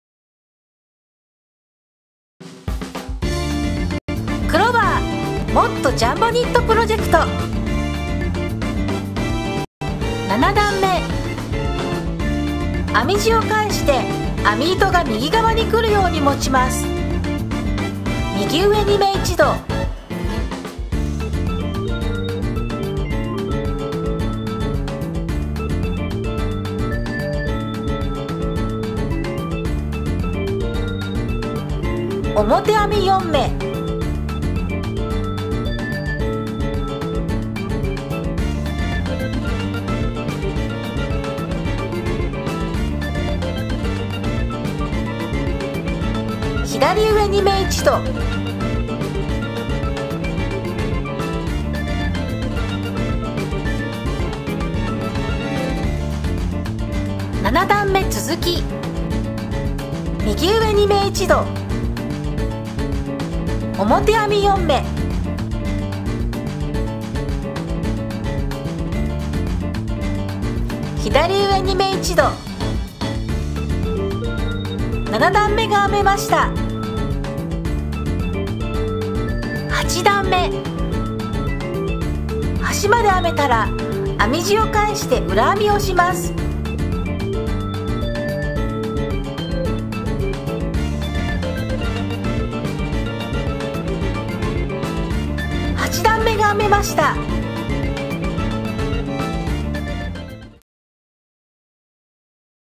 クロバー 2本針ジャンボ〈25mm〉で編む ニットキャップ編み方5 7段め･8段め
クロバー超極太毛糸とジャンボあみ針で
はじめてさんもざくざく編めて楽しいジャンボ編みをはじめましょう。